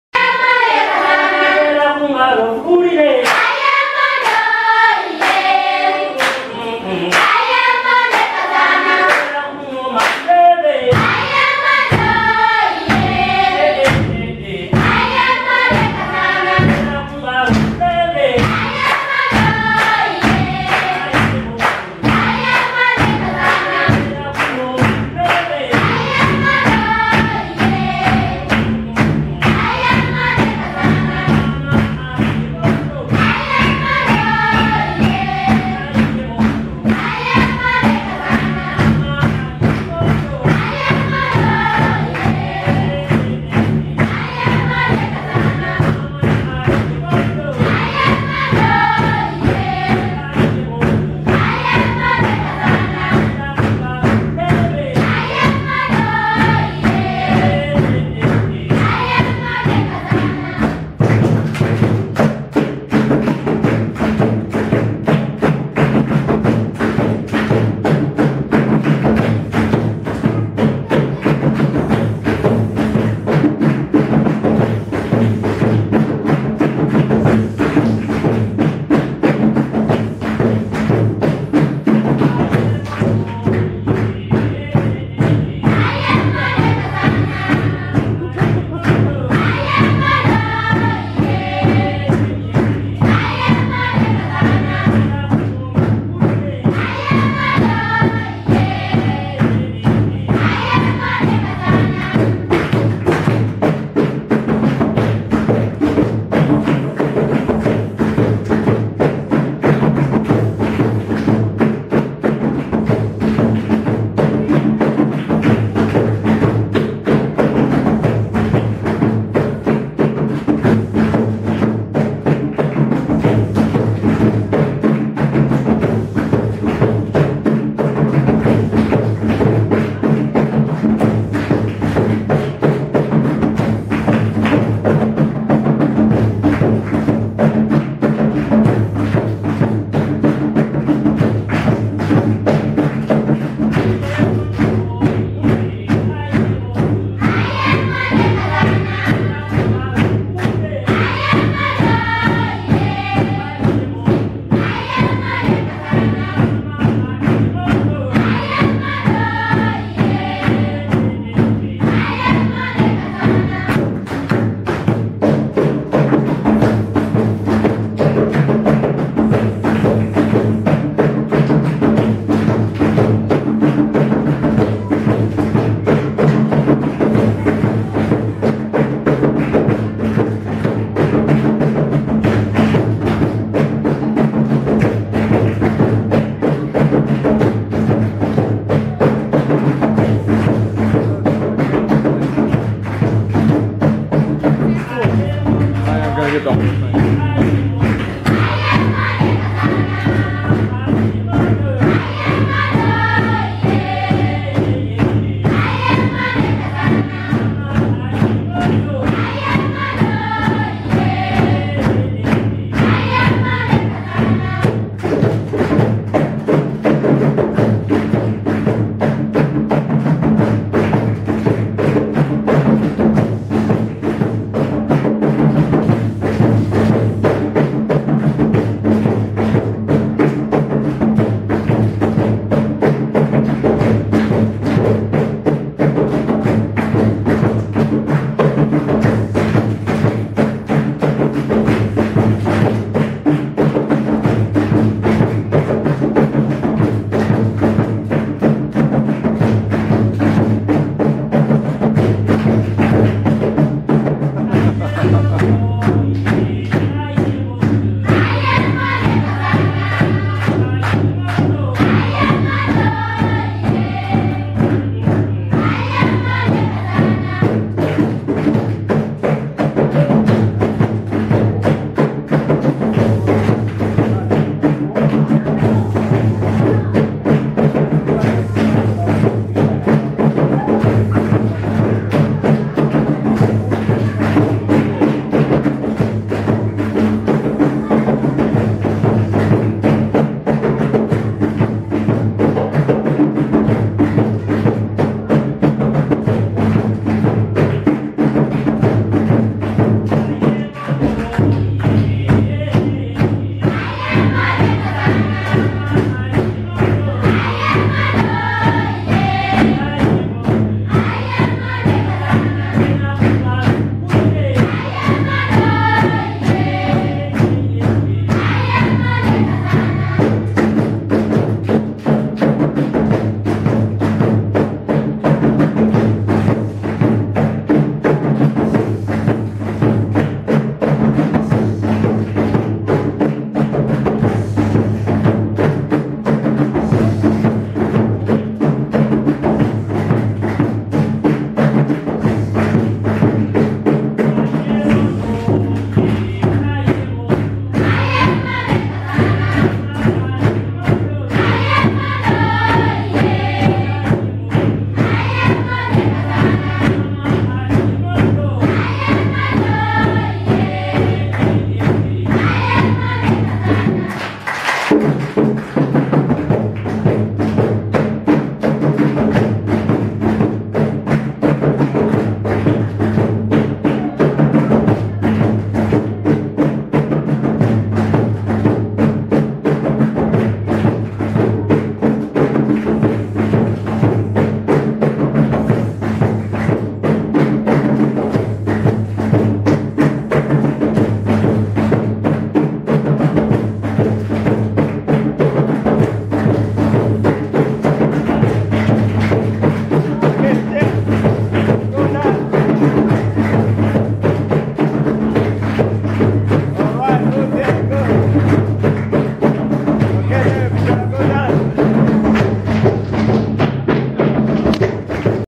African Children Singing and Dancing in Uganda, Africa
An amazing dance for the visitors to Saint Johnson School in Bira, Uganda, Africa. The most insane and beautiful energy!